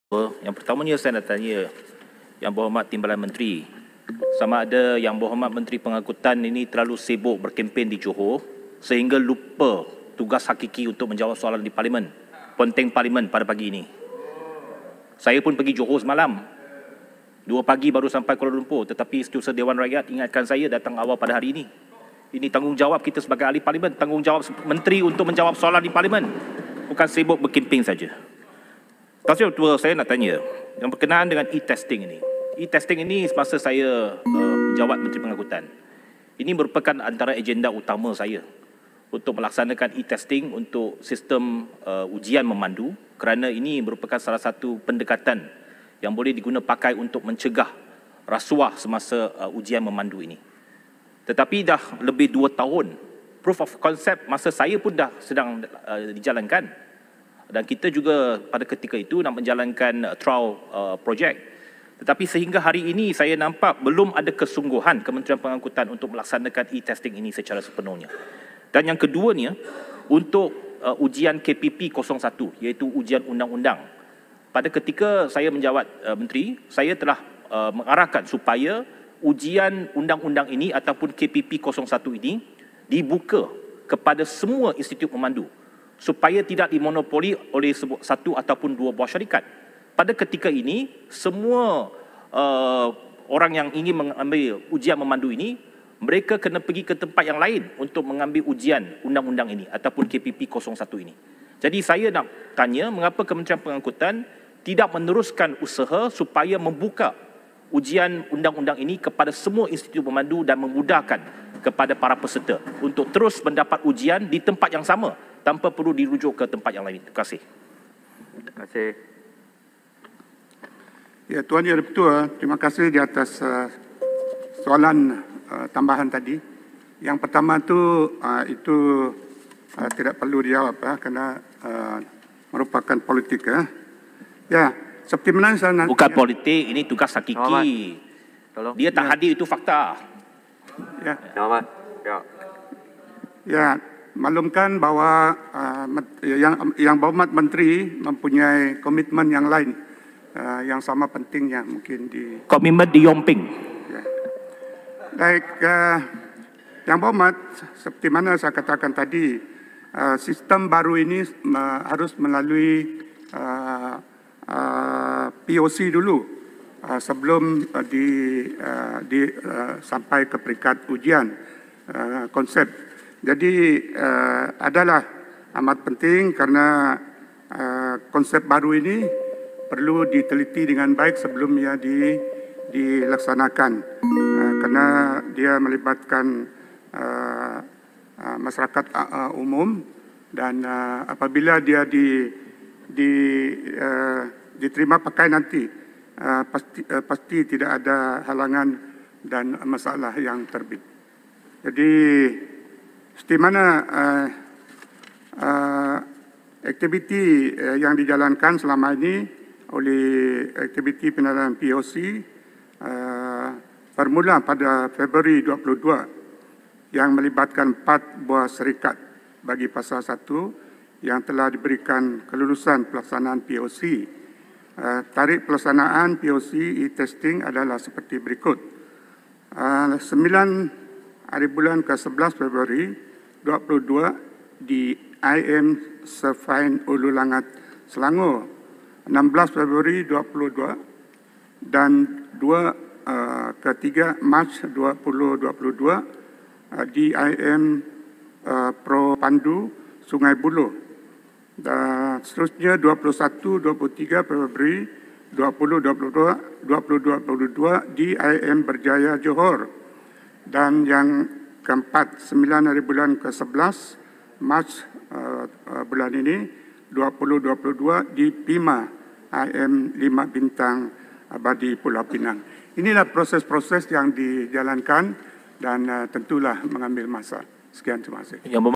Jangan sibuk berkempen sampai ponteng Parlimen - Anthony Loke
"Saya nak tanya Timbalan Menteri, sama ada Menteri Pengangkutan ini terlalu sibuk berkempen (PRN) di Johor sehingga lupa tugas hakiki menjawab di Parlimen," tanya YB Seremban, Anthony Loke ketika membangkitkan isu e-testing.